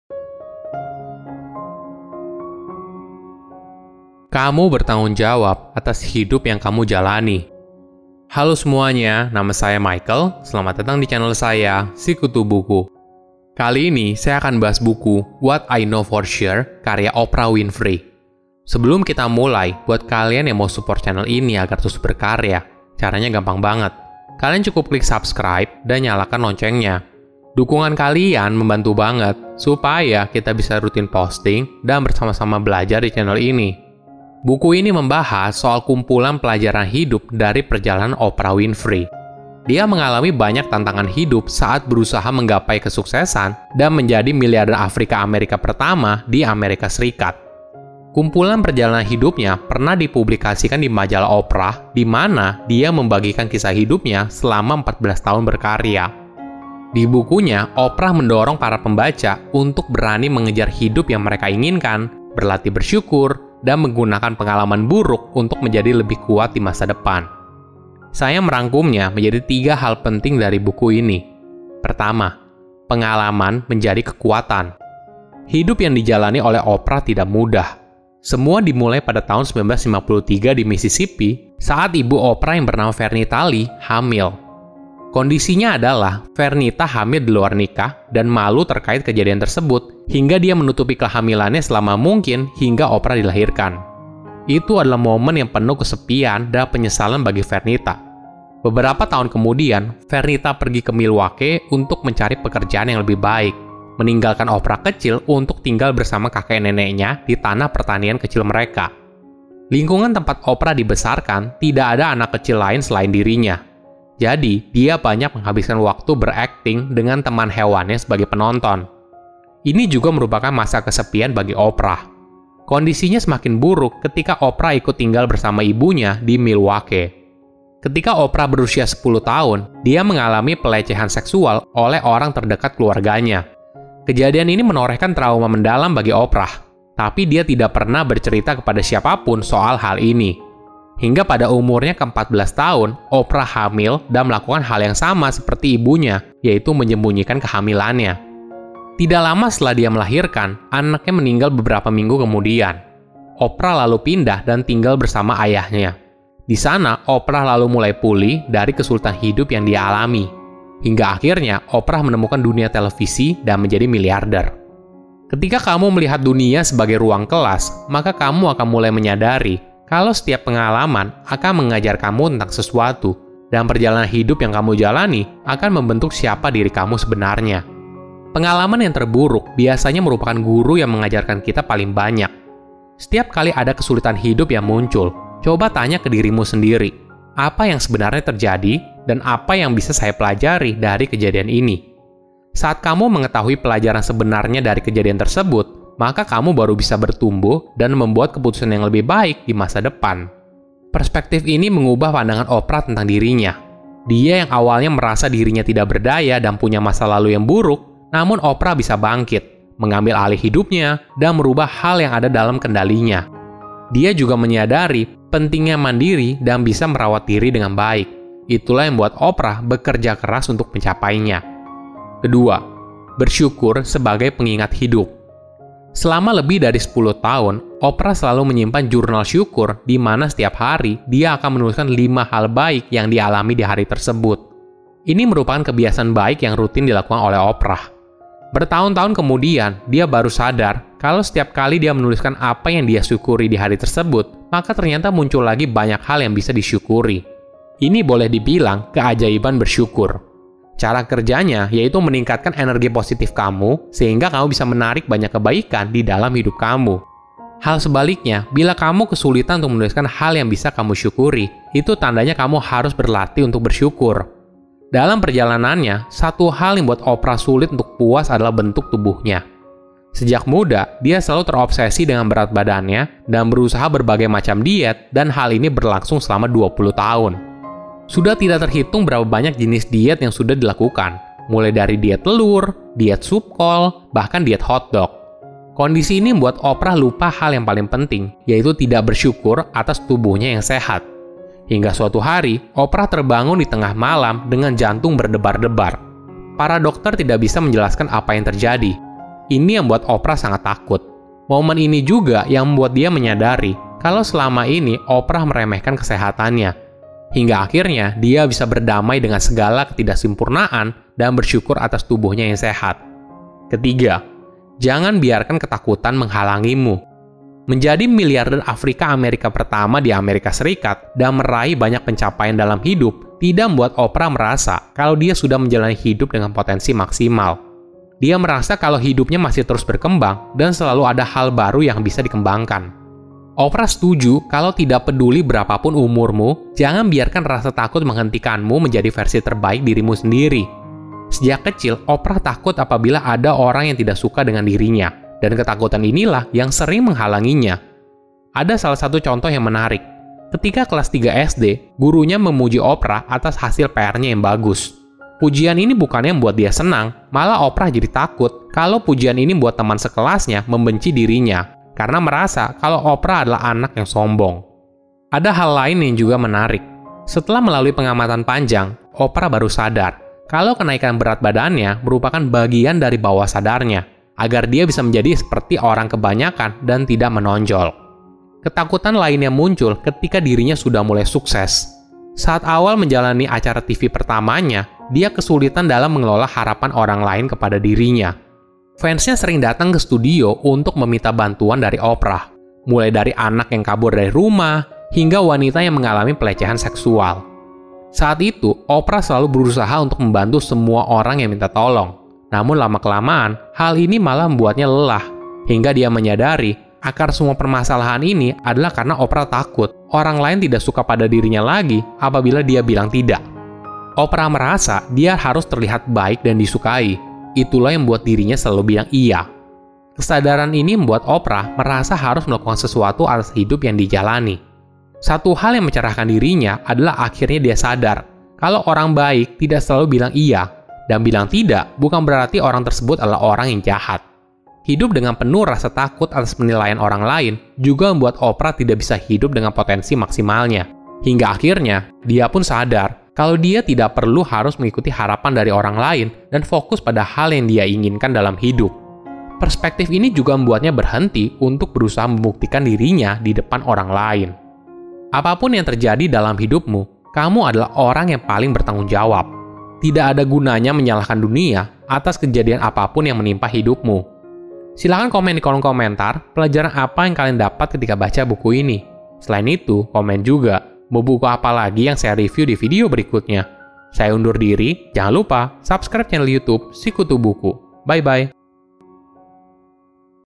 Pelajaran Hidup dari Oprah Winfrey | What I Know For Sure
Saya membahas buku What I Know For Sure karya Oprah Winfrey. Buku ini membahas soal kumpulan pelajaran hidup dari perjalanan Oprah Winfrey. Dia mengalami banyak tantangan hidup saat berusaha menggapai kesuksesan dan menjadi miliarder Afrika-Amerika pertama di Amerika Serikat. Kumpulan perjalanan hidupnya pernah dipublikasikan di majalah Oprah di mana dia membagikan kisah hidupnya selama 14 tahun berkarya.